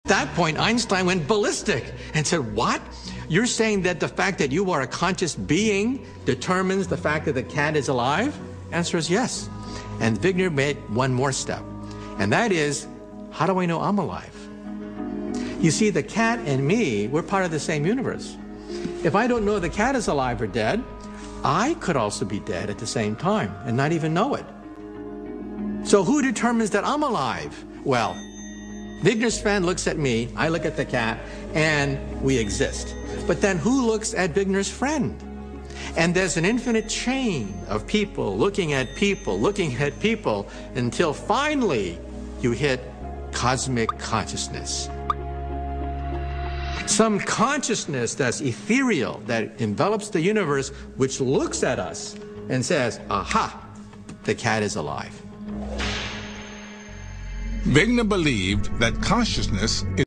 Michio Kaku On Quantum Physics and Consciousness
Michio Kaku explains consciences and quantum physics